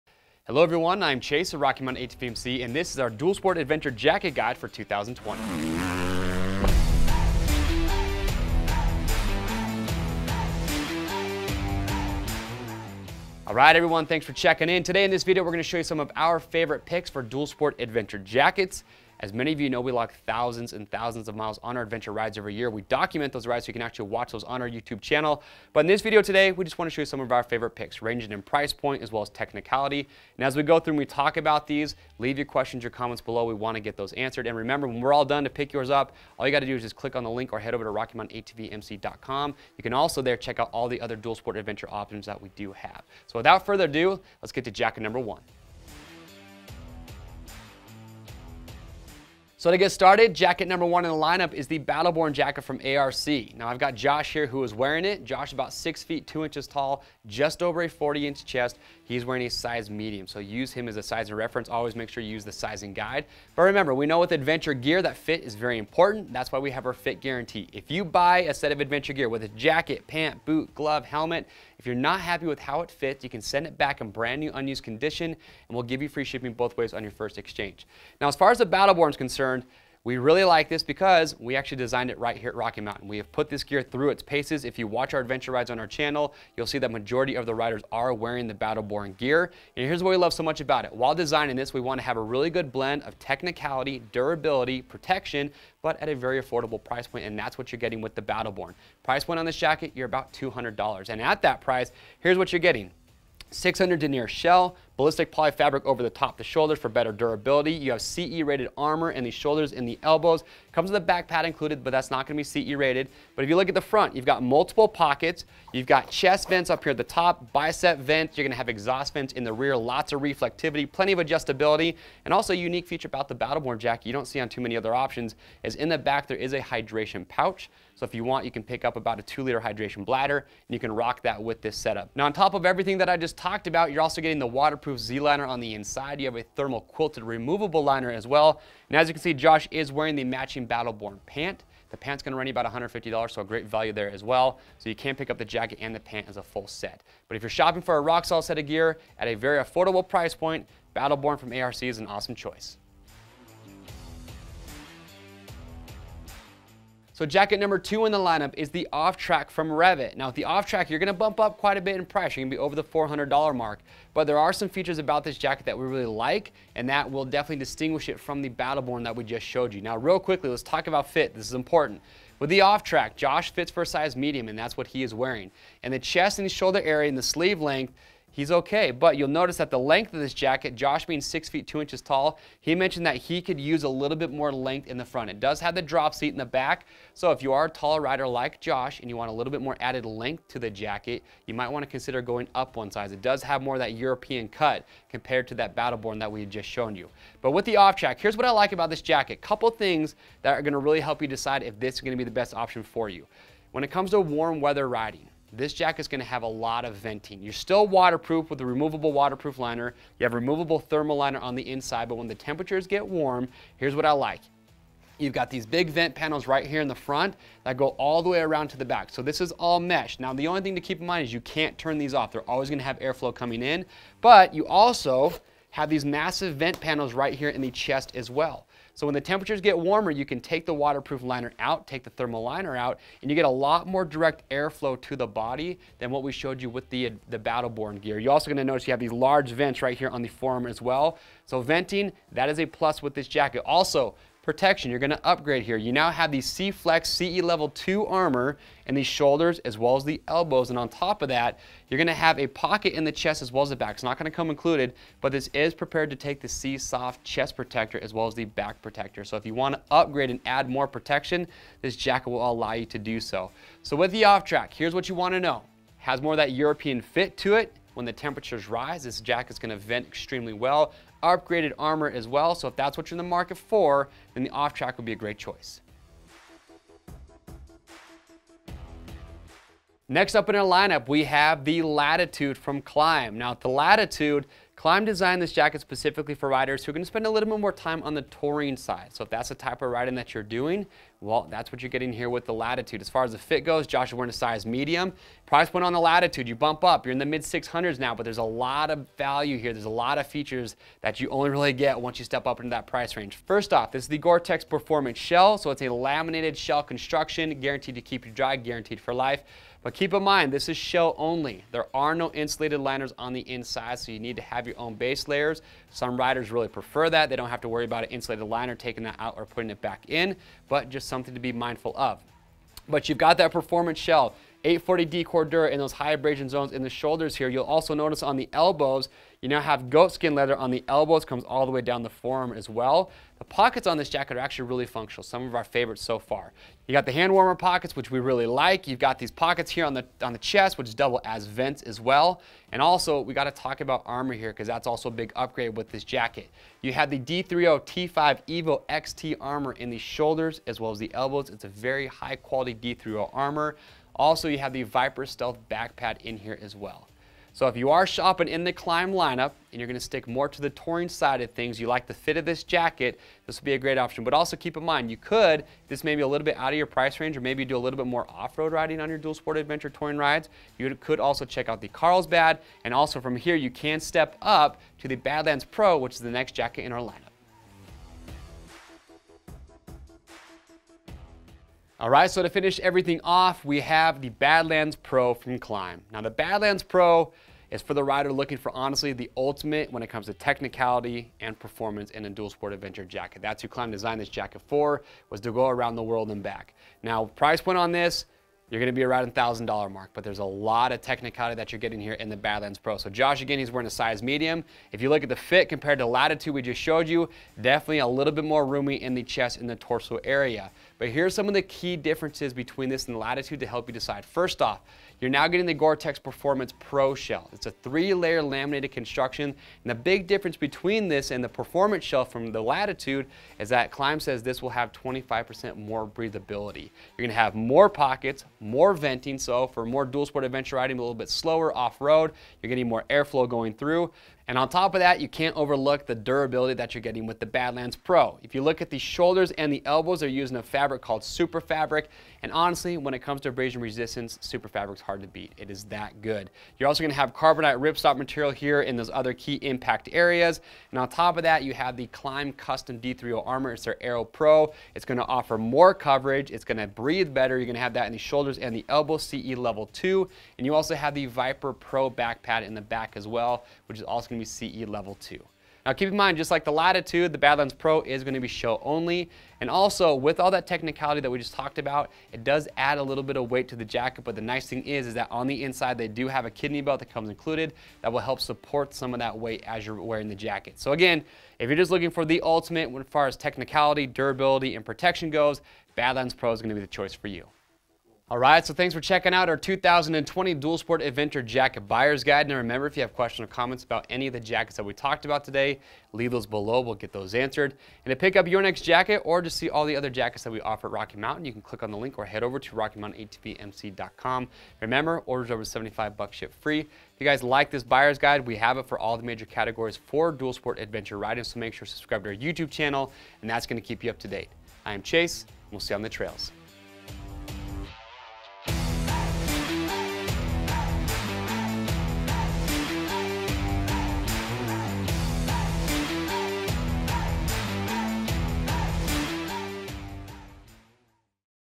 Best ADV/Dual Sport Motorcycle Jackets | 2020
Stay comfortable and protected with the best adventure and dual sport motorcycle jackets on the market. With features like waterproof liners, ventilation where you want it, and tough, advanced fabrics where you need it, our top adventure jackets are sure to keep you riding no matter what mother nature throws your way. So, tune in as Chase goes over our top adventure and dual sport motorcycle jackets for 2020.